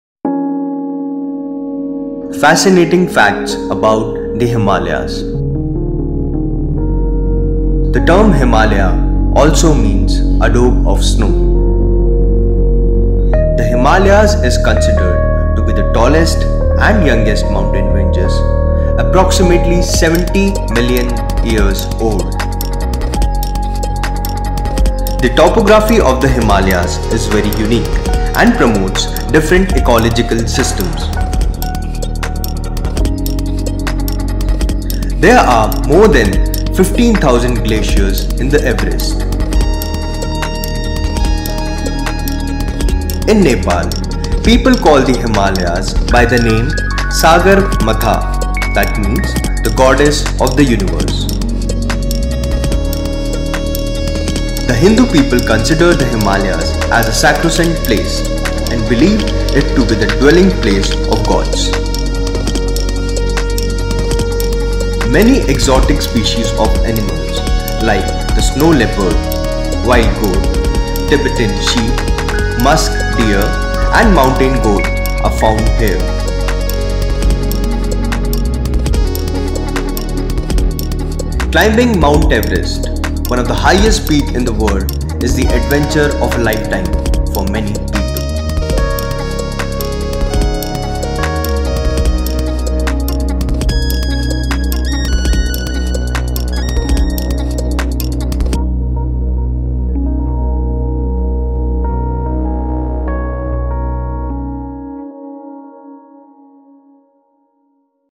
Fascinating Facts about the Himalayas | travel tips | video | advice
Most people are aware of the fact that the snow-clad Himalayas is one of the highest mountain chains, but you will be surprised to know that this peak grows approximately 20mm every year. It is not the only interesting fact about the Himalayas, there are plenty more mentioned in this video.